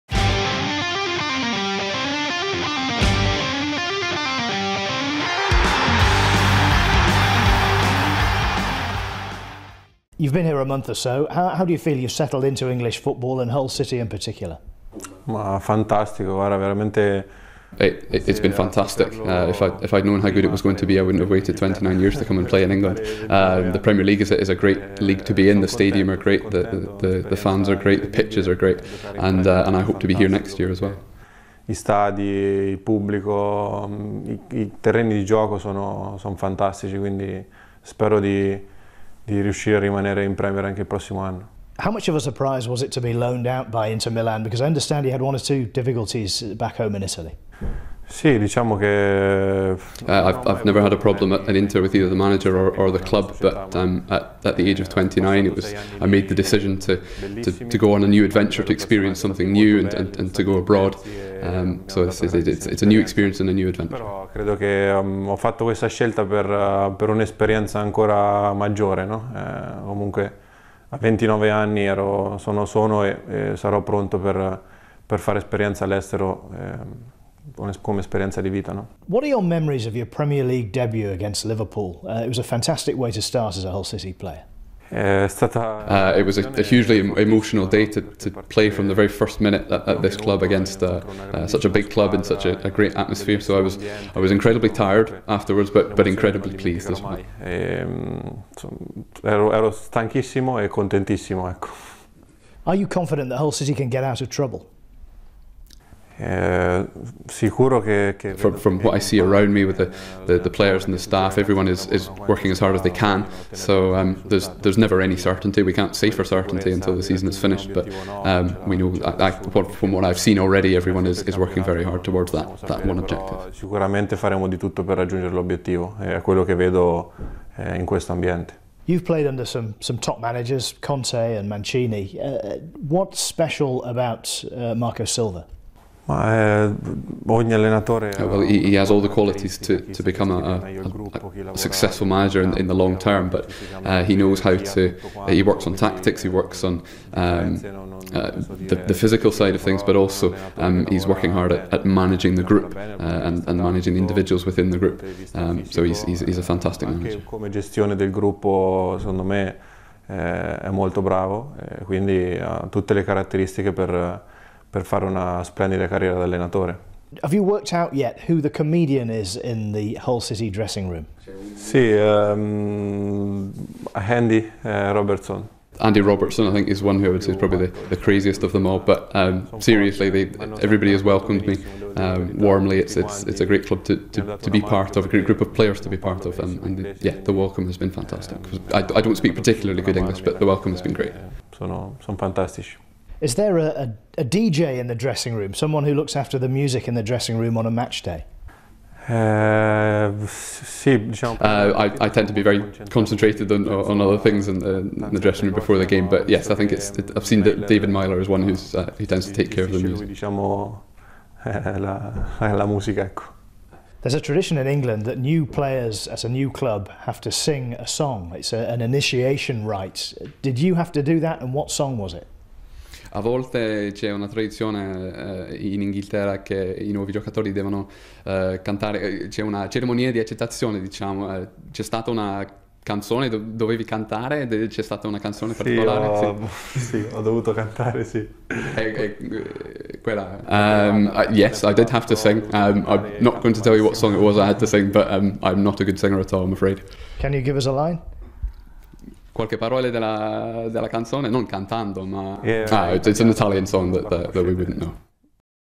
Interview | Andrea Ranocchia on Life at the Club & His Move to the Premier League
Centre Back Andrea Ranocchia sits down with Premier League Productions to talk about his time so far at the club.